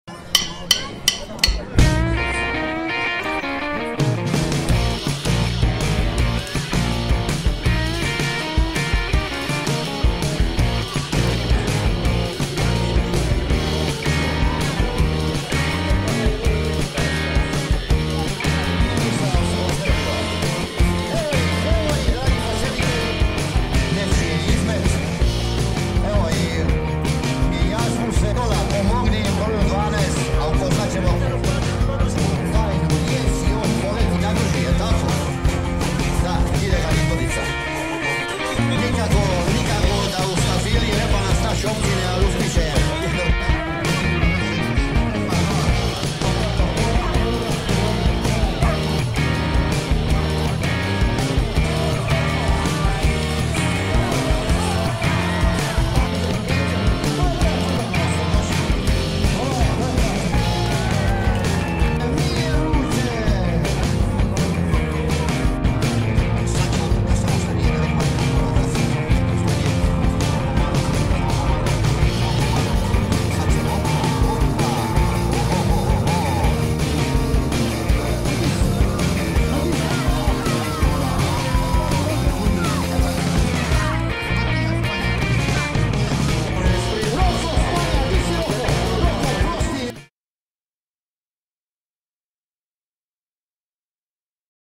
53. utrka tovara u Tribunju | PIXSELL PLUS
U Tribunju je održana tradicionalna 53. utrka tovara na kojoj je pobjedio Ivan Stipaničev na tovaru Rebalans.